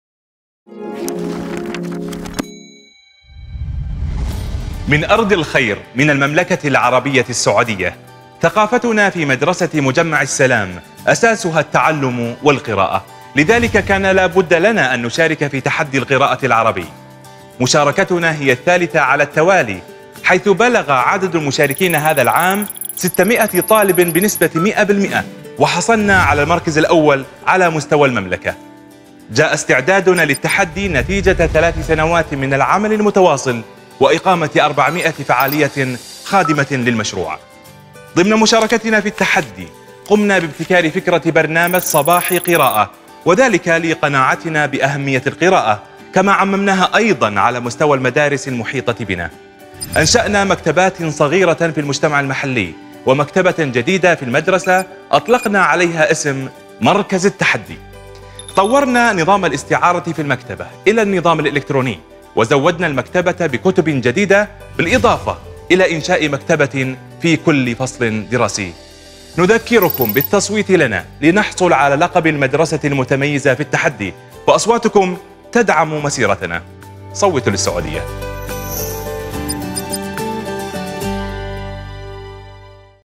مدرسة مجمع السلام - السعودية - المرشحة لجائزة المليون درهم
تتنافس خمس مدارس تأهلت للتصفيات النهائية على لقب المدرسة المتميزة، والجائزة النقدية المُقدرة بمليون درهم إماراتي في تحدي القراءة العربي 2018، من أصل 52 ألف مدرسة مشاركة في التحدي بدورته الثالثة
تشارك هذا العام مدرسة "مجمع السلام" من المملكة العربية السعودية، عبر ابتكار فكرة برنامج "صباحي قراءة" التي شهدت نجاحاً واسعاً، وتم نشرها على مستوى المدارس المحيطة والمنطقة
 شاركوا الآن وصوتوا للمدرسة الأفضل عبر الموقع
تحدي القراءة العربي هو أكبر مشروع عربي أطلقه صاحب السمو الشيخ محمد بن راشد آل مكتوم، نائب رئيس دولة الإمارات العربية المتحدة رئيس مجلس الوزراء حاكم دبي، لتشجيع القراءة لدى الطلاب في العالم العربي